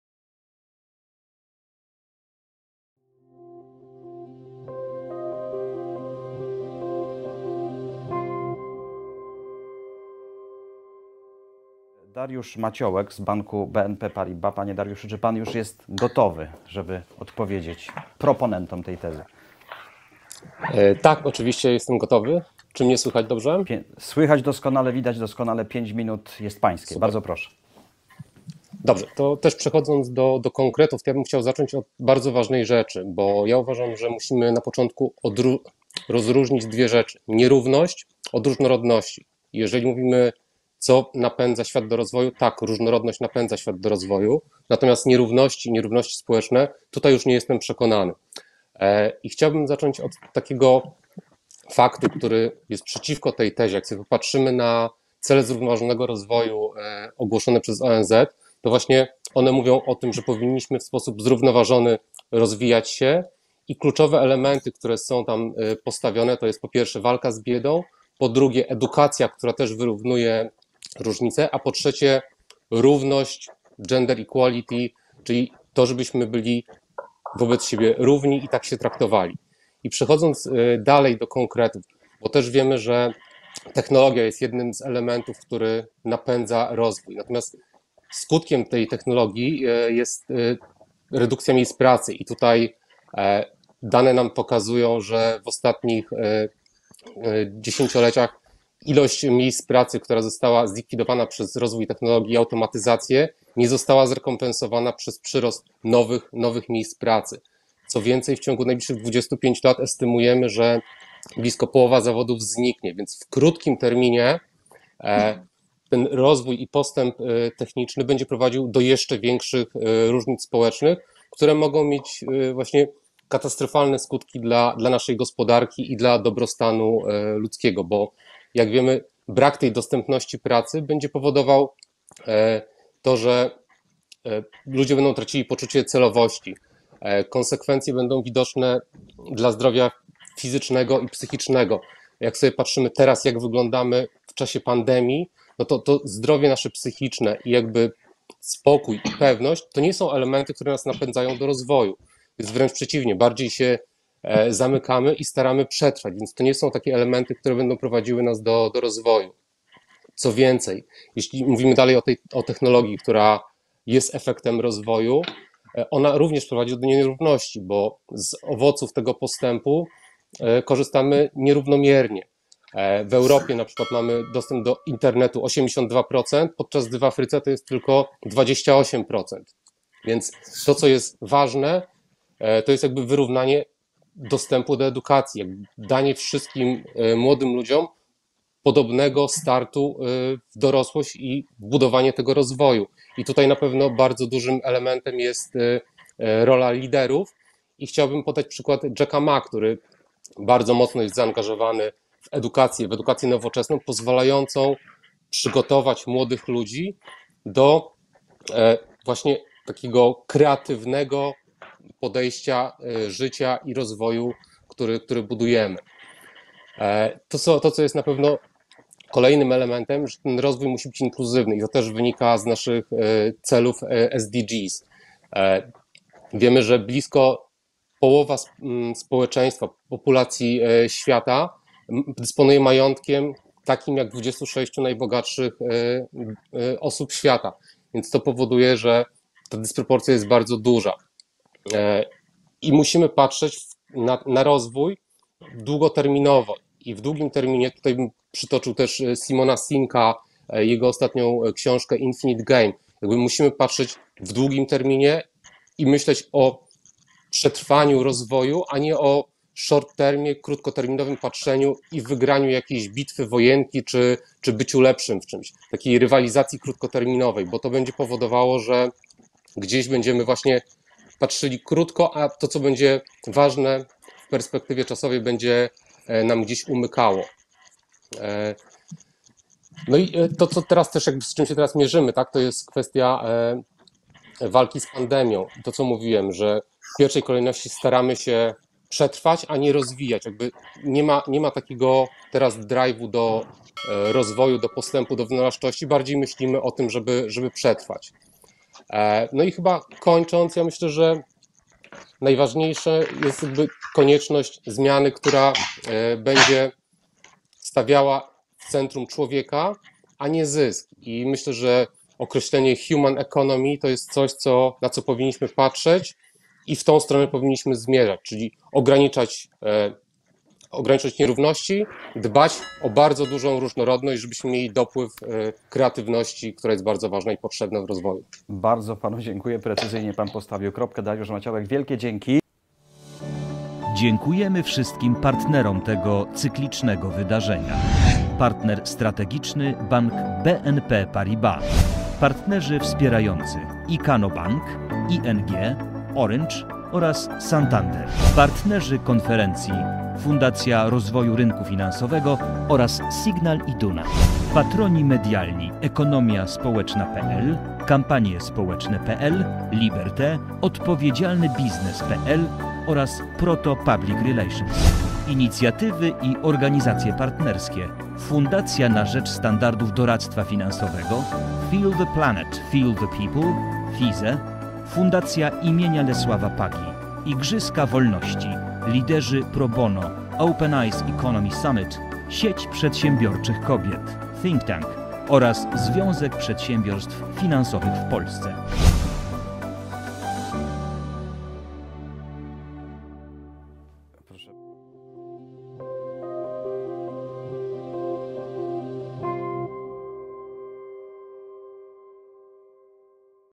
Dariusz Maciołek - Debata: Nierówności społeczne napędzają rozwój świata
Fragment debaty warszawskiej w stylu oksfordzkim: Nierówności społeczne napędzają rozwój świata.

Relacja z VIII Konferencji Nienieodpowiedzialni „Zazdrość, nierówność, polaryzacja... przyszłość?”